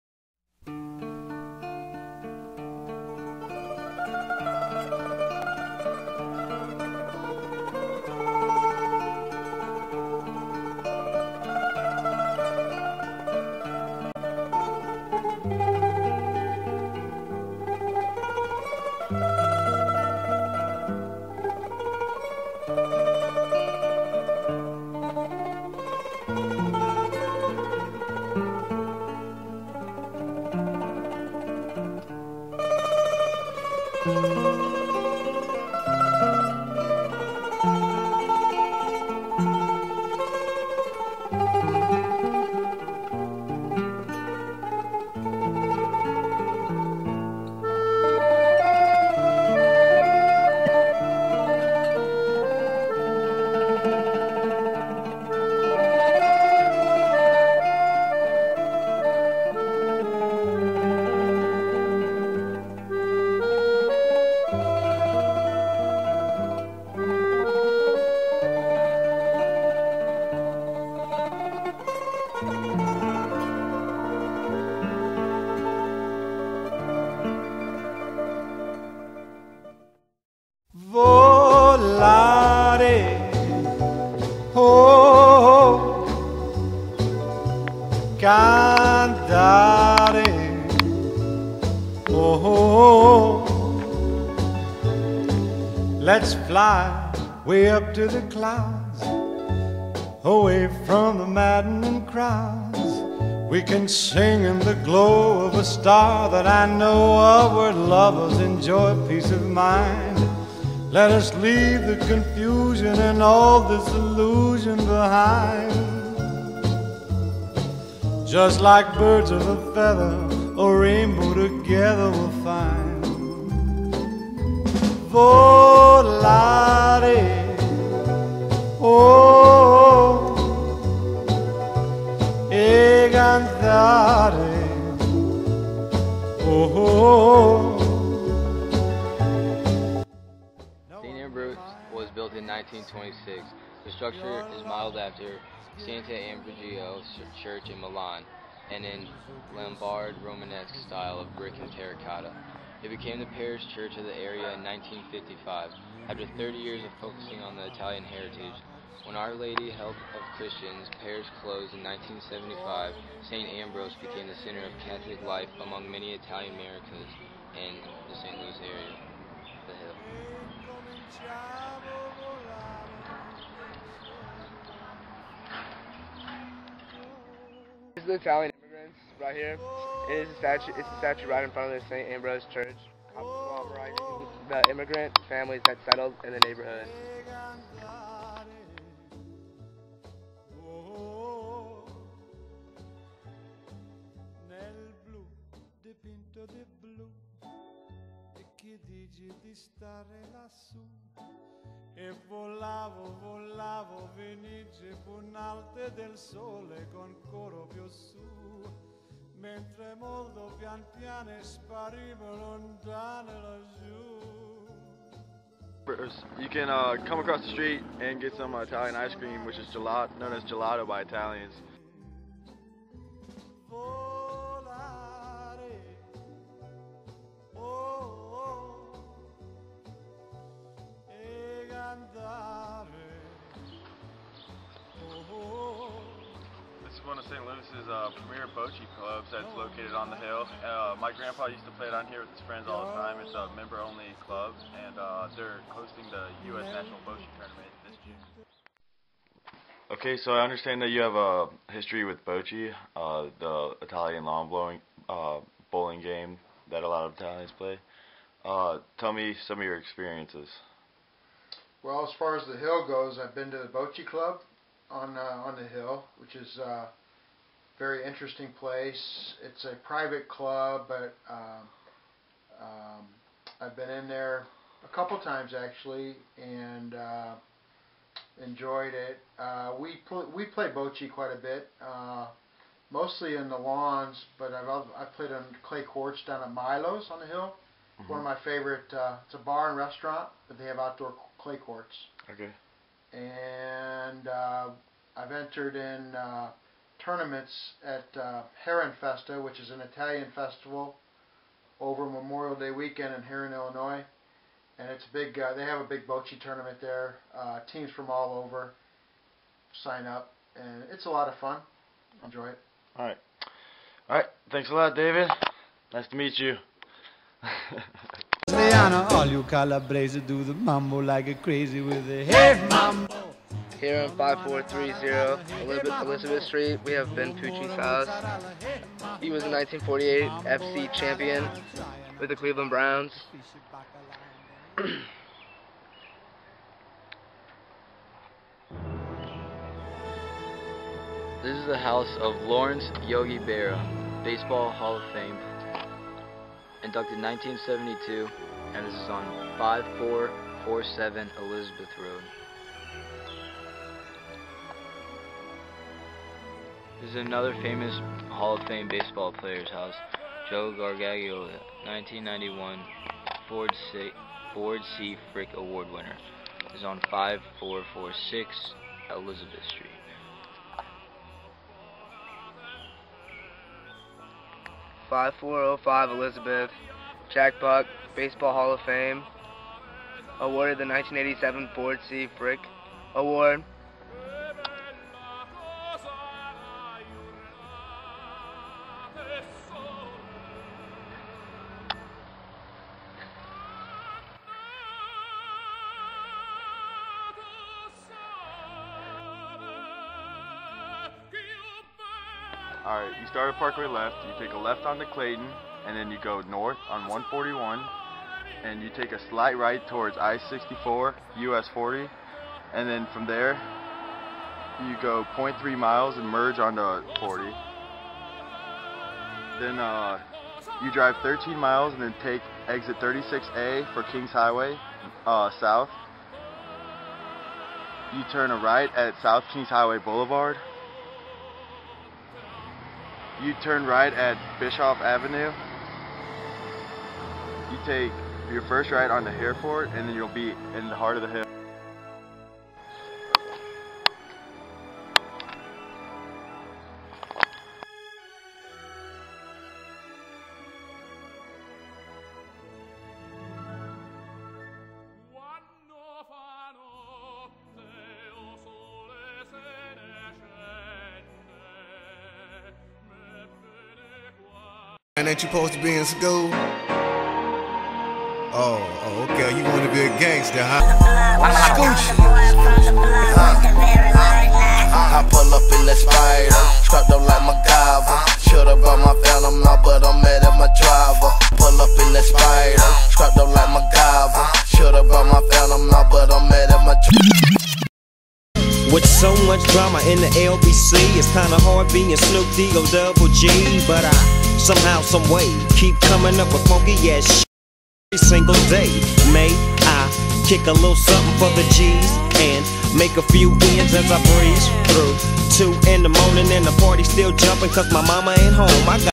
The Hill Saint Louis
A view of the hill. la montana near downtown Saint Louis